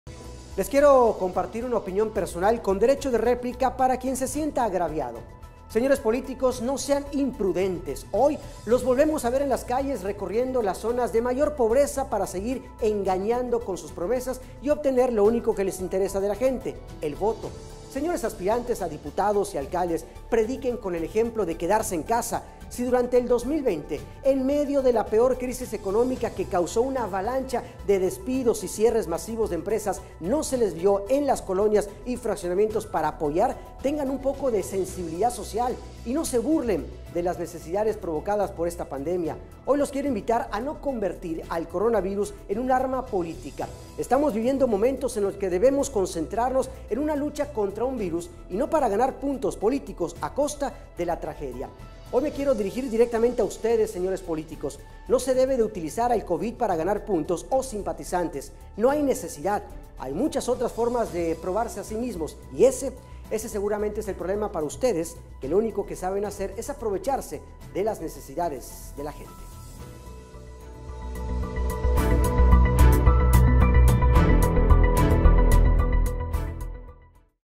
EL COVID-19: UN ARMA POLITICA PARA CONSEGUIR VOTOS
Editorial 2021 año electoral donde ahora sí los políticos bajan al pueblo... ¿Dónde estuvieron durante la peor parte de la pandemia?